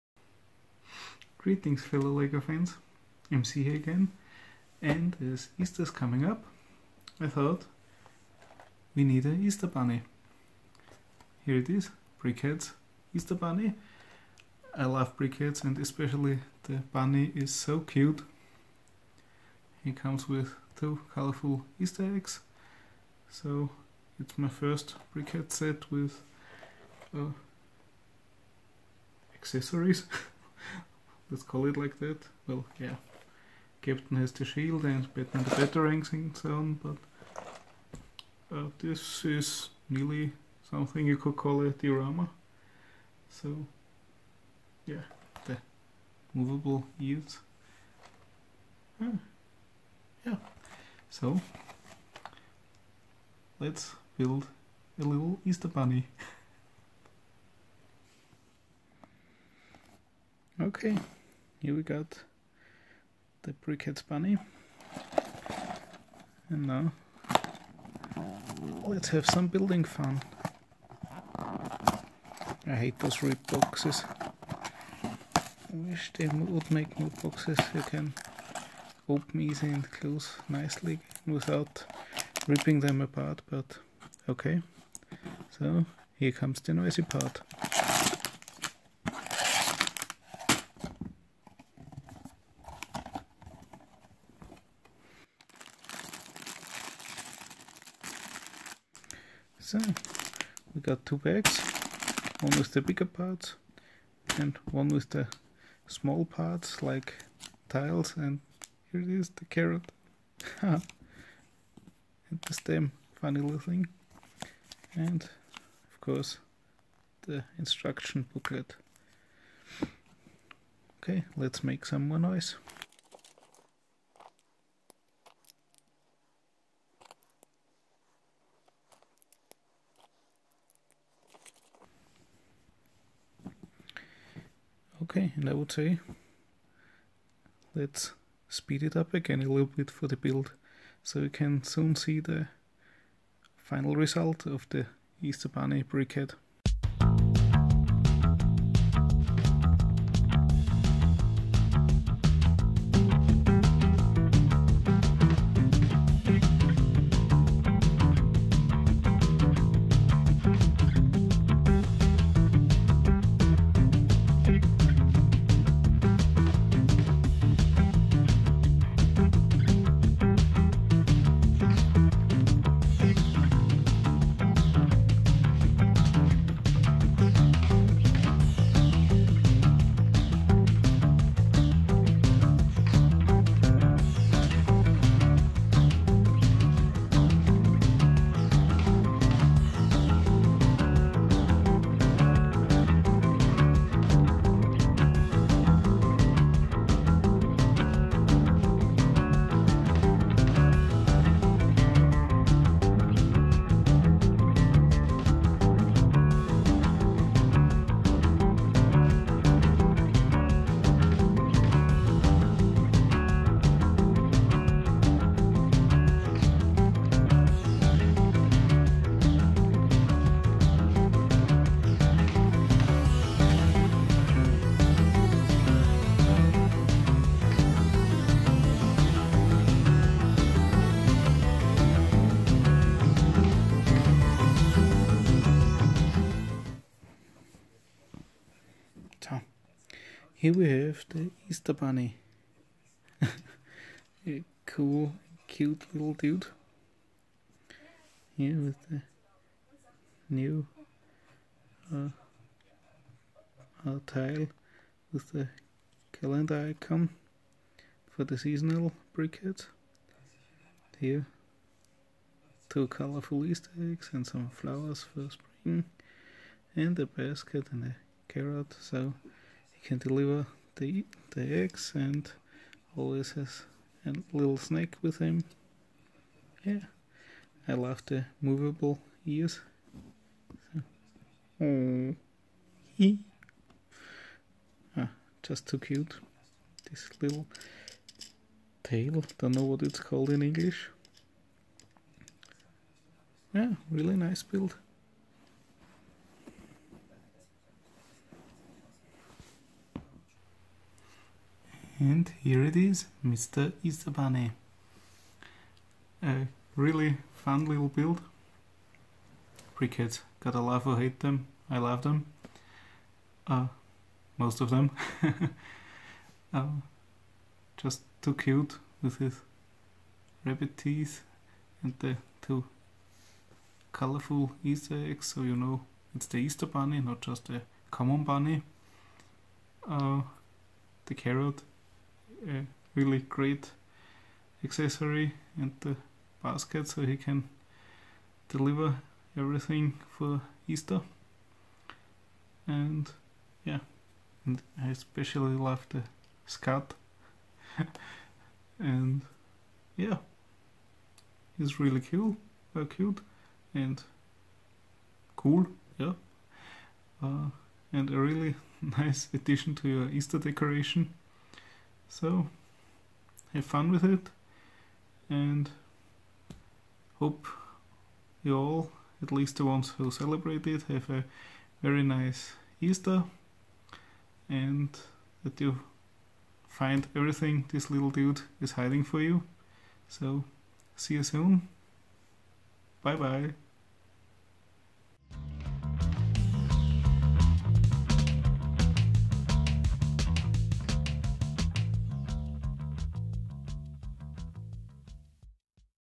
Happy Easter with the Brickheadz Bunny
As I have mentioned before I love Brickheadz. So it is obvious that I would get the Easter Bunny from Lego’s Brickheadz.
It is a really cute build with two colorful easter eggs, a carrot and a basket.
I love the moveable ears, the teeth and the sweet little scut.
The bunny is a nice addition to every Lego fan’s easter decoration.

Filmed with my Samsung Galaxy S7.